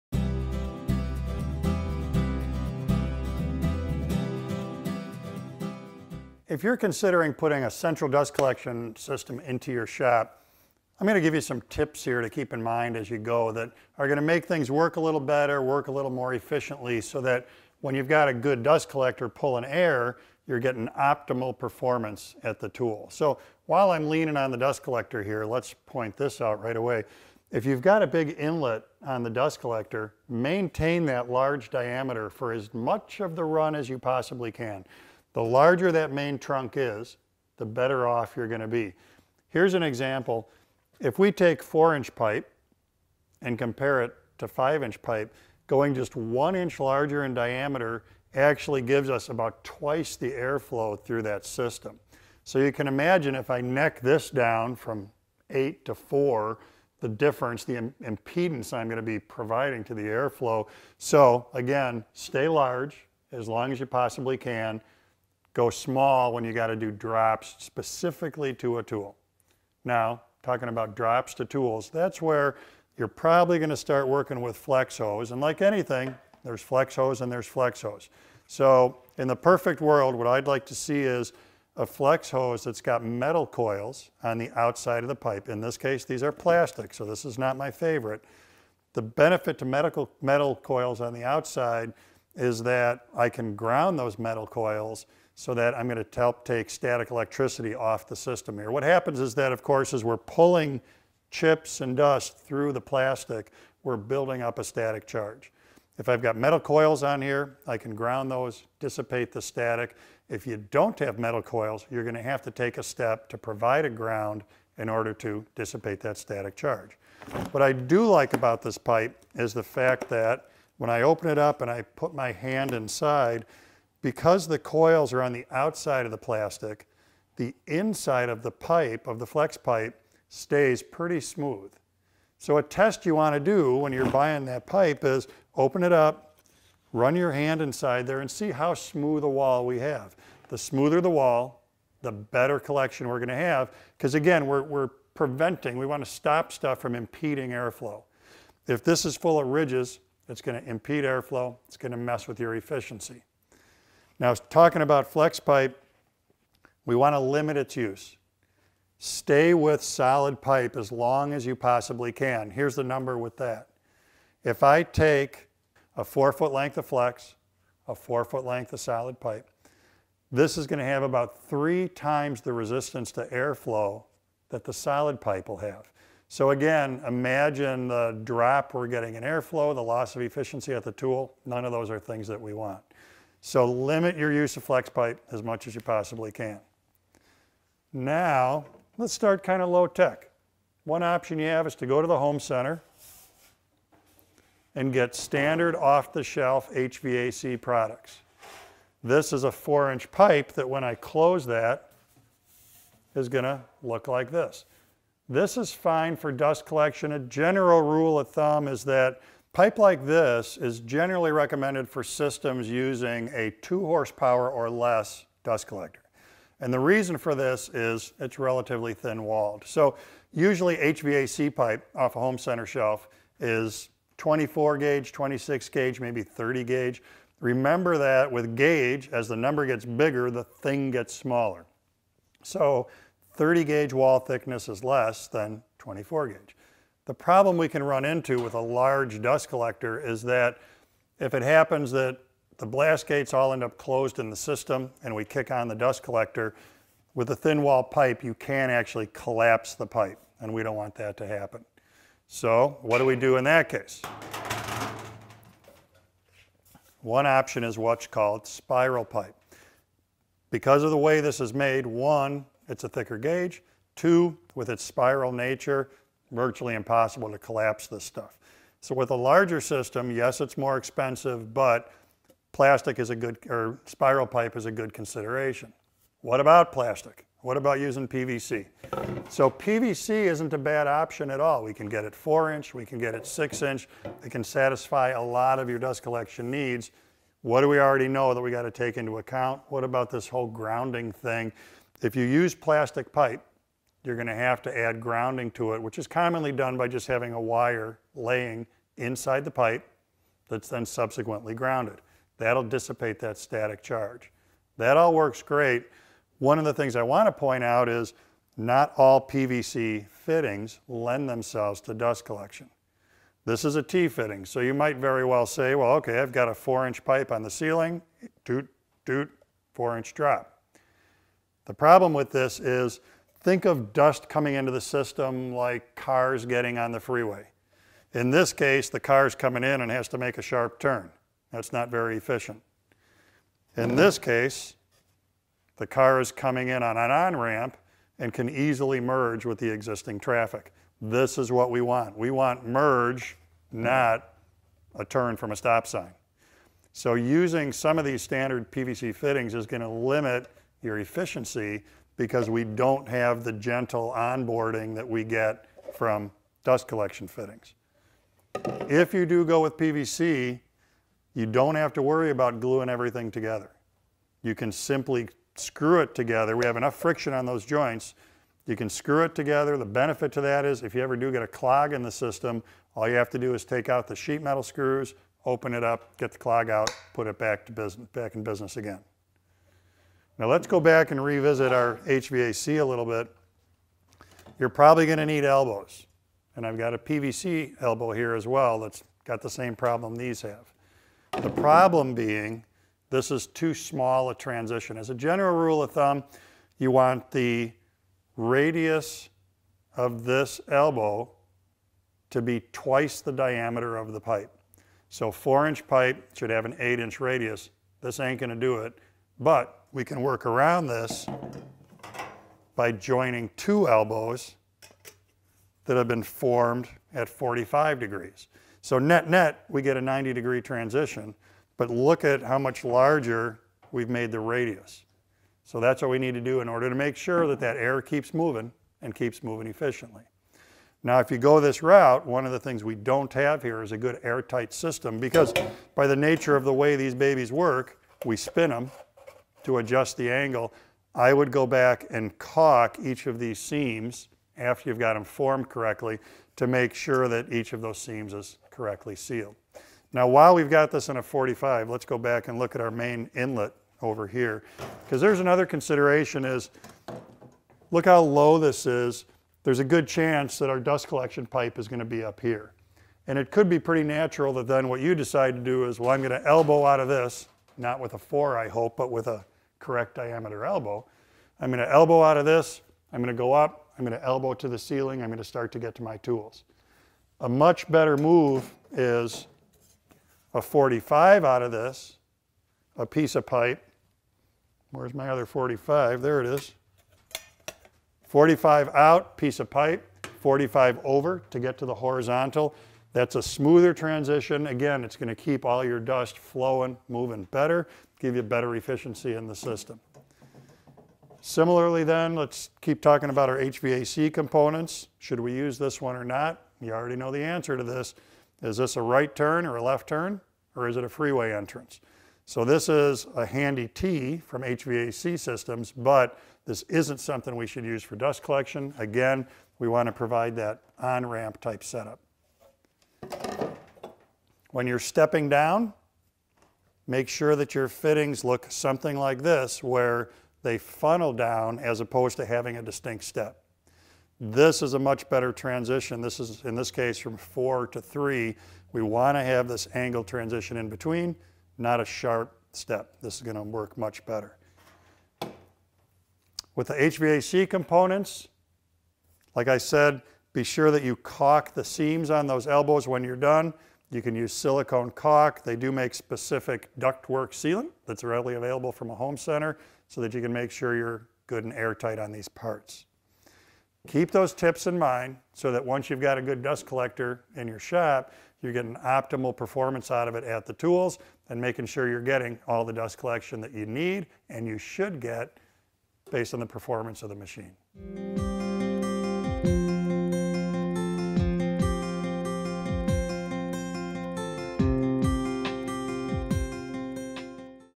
Tips for Central Dust Collection
🔨 Sign up for the free WWGOA newsletter to find the latest woodworking tips and techniques right in your inbox.

Once you’ve invested in a dust collector for your shop and are considering setting it up as a central system, be sure you educate yourself about the best practices for piping the system in. This video will go a long way toward helping you keep your system efficient.

It’s in the details:
Creating an efficient central dust collection system is all about the little things. You’ve got to pay attention to the small details to get the system to do what you want it to do. What type of fittings should you buy? Can plumbing fittings be substituted for dust collection fittings? Should you seal the joints? Where should you use flex hose instead of rigid pipe? What type of flex pipe is best? What’s the best way to reduce the diameter of the pipe? This video provides answers to these questions, and will help you make your system efficient.

Elbows, T’s and Y’s:
Woodworkers often use the wrong fittings, and this adversely affects their dust control system. Elbows need to be the correct radius, based on the diameter of the pipe they’re connected to. T’s aren’t great, Y’s are a better choice. The video explains why.

More tips:
If you’re looking for more help on installing a central dust collection system, and keeping it efficient, have a look at Paul Mayer’s article that provides a simple approach for dust collection ducting.

For more information on Laguna dust collectors and Laguna cyclone dust collectors